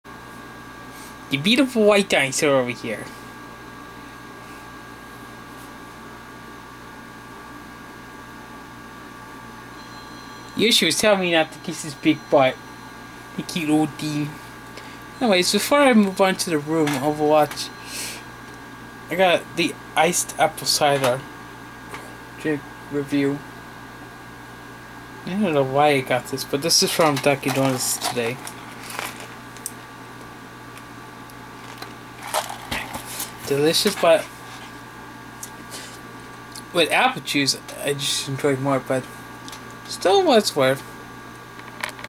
Drink Review For Iced Apple Cider from Dunkin Donuts
This is my Drink Review For the Iced Apple Cider from Dunkin Donuts